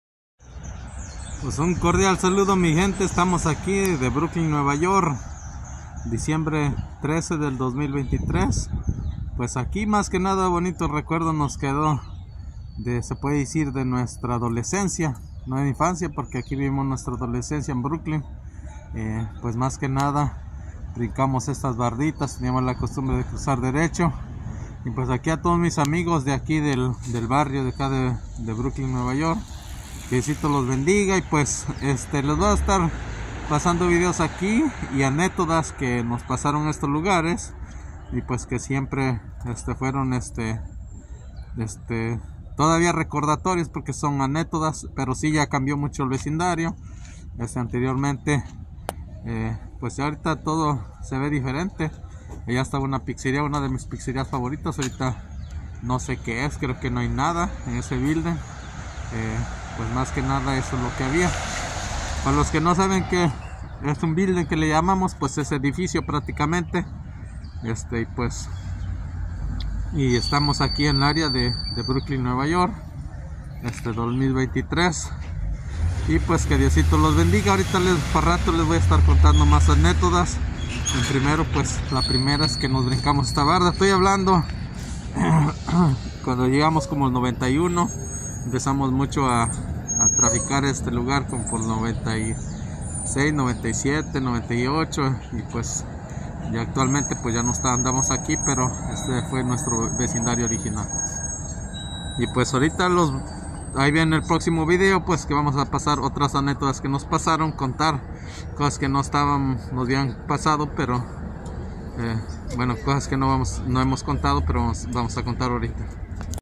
SUNSET PARK, BROOKLYN, NY recorriendo el lugar donde vivimos nuestra adolecencia Diciembre 13, 2023
Brooklyn cambio para lo bueno